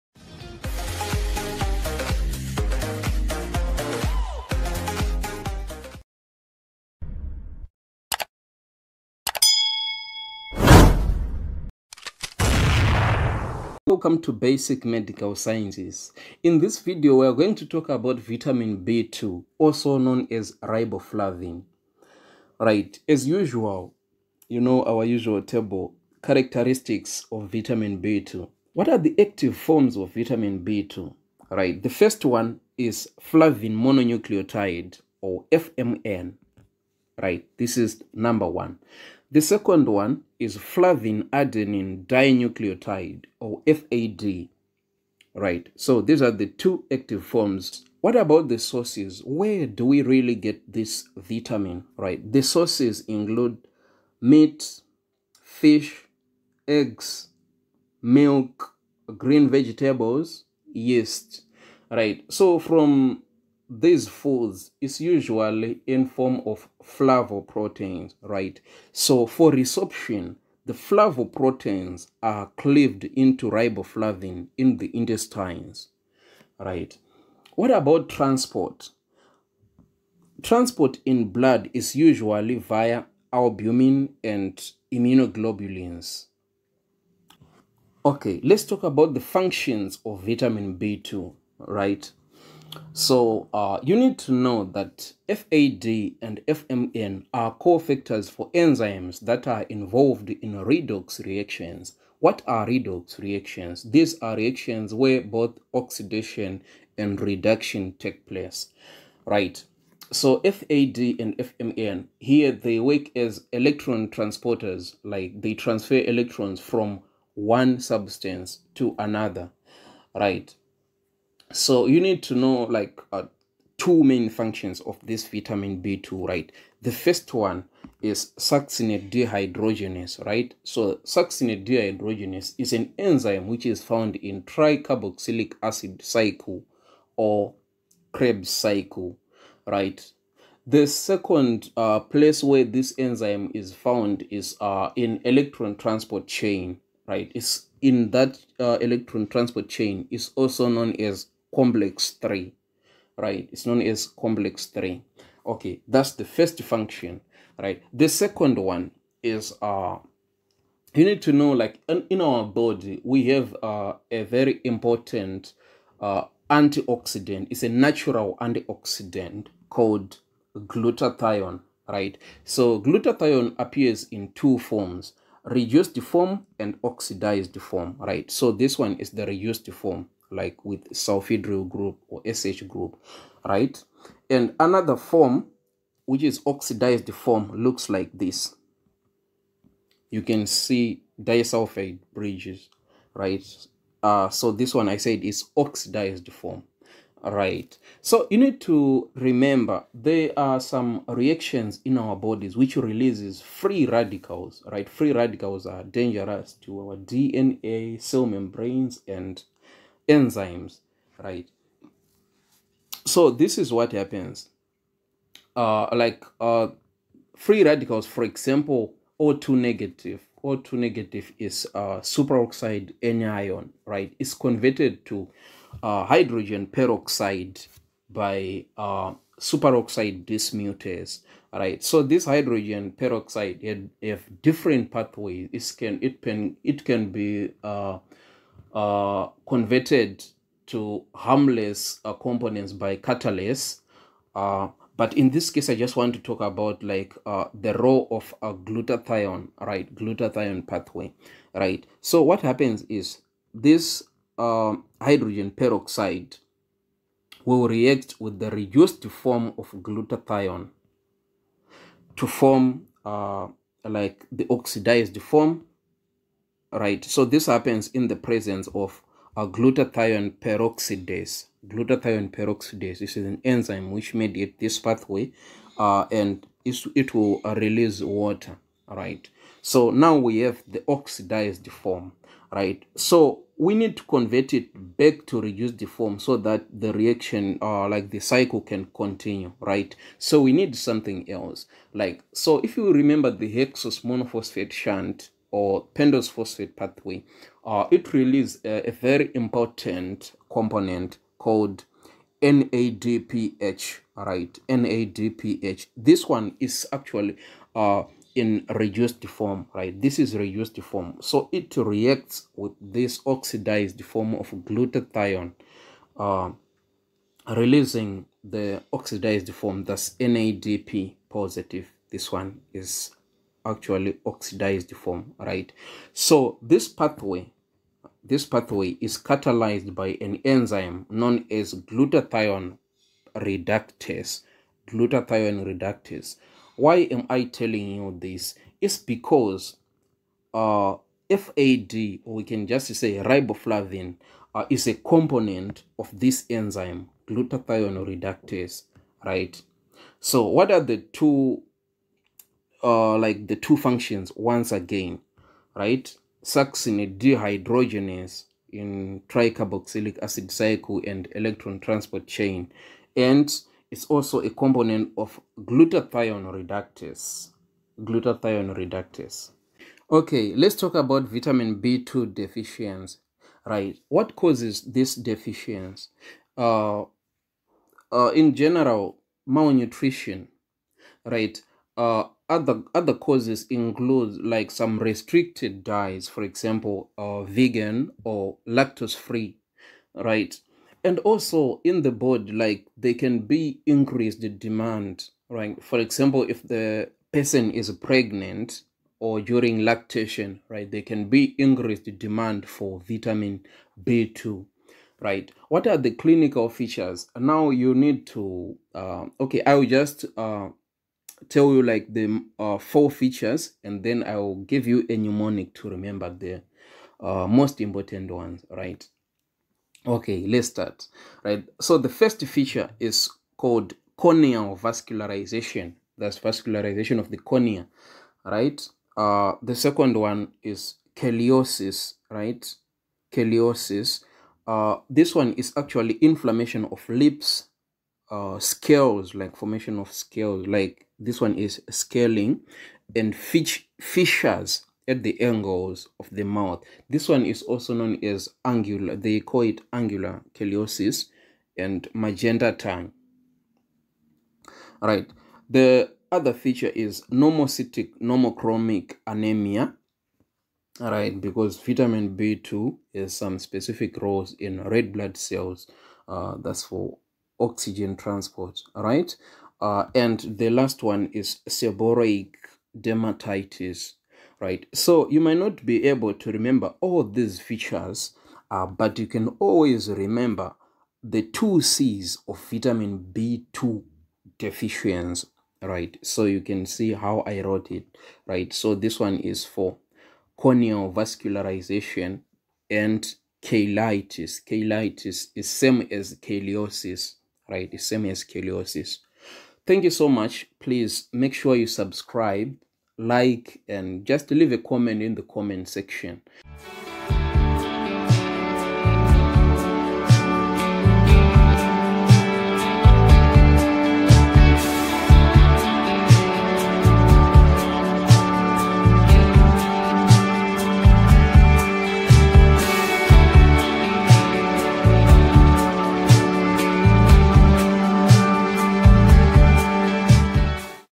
Vitamin B2 (Riboflavin)- sources,functions, deficiency
In this video, Phil will be presenting everything you need to know about Vitamin B2(Riboflavin)
Its sources, functions and deficiency